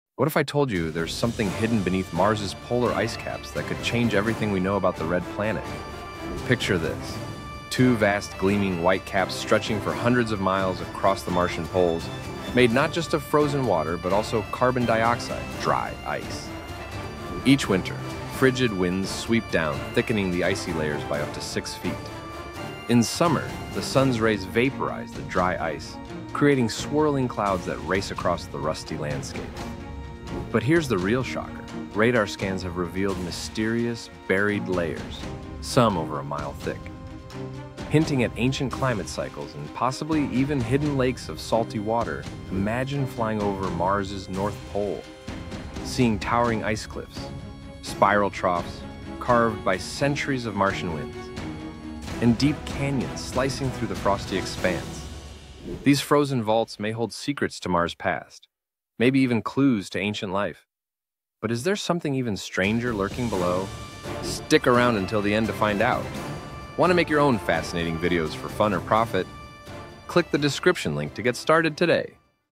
Secrets Beneath Mars’ Mysterious Polar Caps
What’s really hiding under Mars’ icy poles? Uncover mind-blowing secrets about the Red Planet’s frozen mysteries!